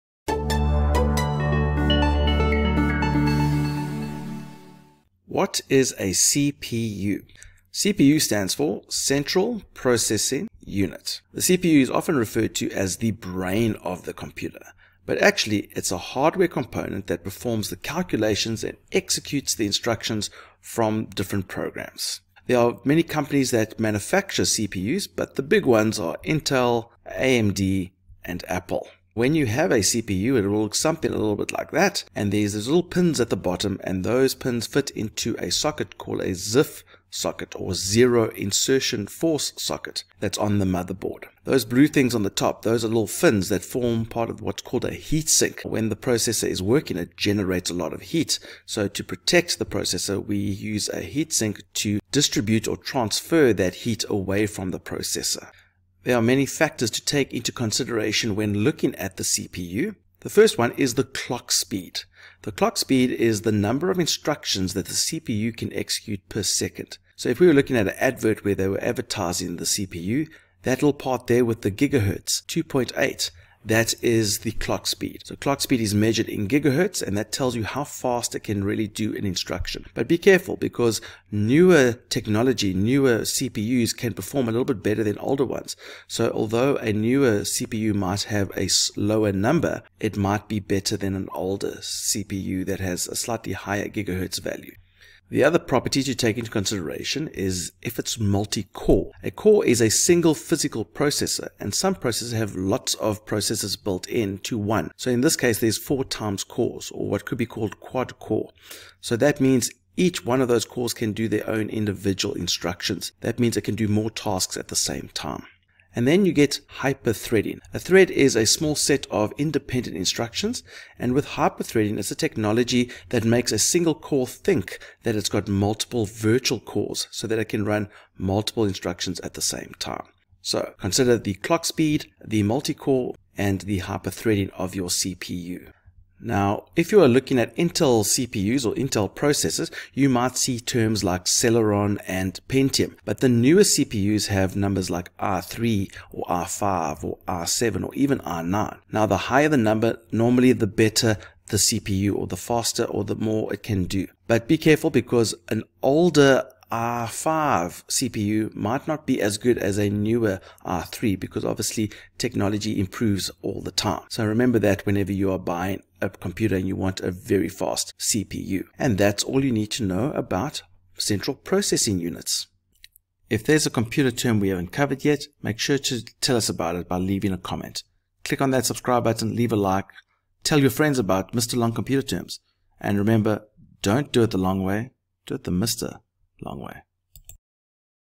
Mr Long Computer Terms | What is a CPU?
This video explains what is a Central Processing Unit, their specifications and details about the manufacturers.

Category: Hardware